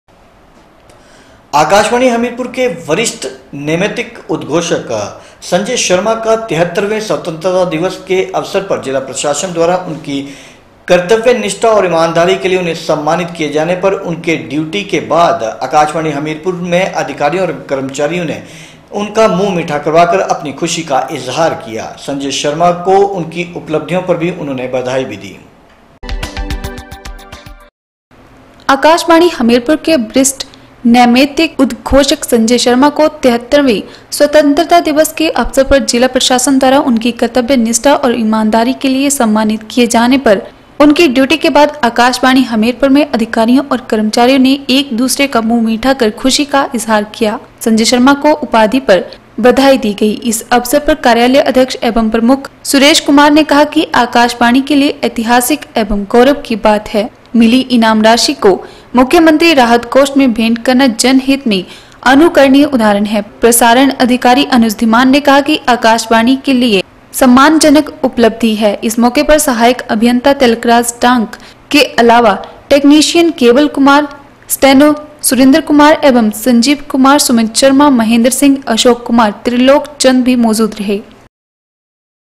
22 AUG N 6 Congratulations to Sanjay Sharma on being presented. hamir[pur aakhasvani
आकाशवाणी  हमीरपुर   के  वरिष्ठ   नैमेतिक  उदघोषक  संजय शर्मा  को 73 वें स्वतंत्रता दिवस के अवसर जिला प्रशासन द्वारा उनकी कर्तव्य निष्ठा और ईमानदारी के लिए समानित किए जाने पर  उनकी ड्यूटी के  बाद  आकाशवाणी  हमीरपुर  में  अधिकारियों और कर्मचारियों ने एक दूसरे मुंह मीठा कर खुशी का इजहार किया गया। संजय शर्मा को उपलब्धि पर बधाई दी।